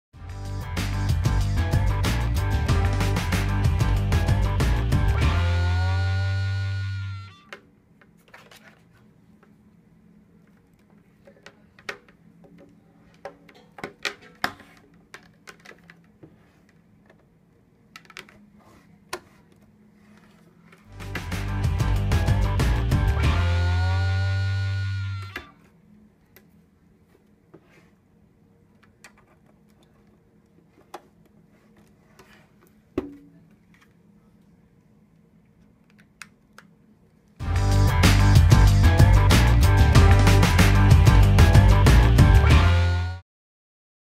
GoDex Printer - Keyboard & Power Cords
Just pulled your printer out of the box? We show you how to get your printer plugged in and ready to print.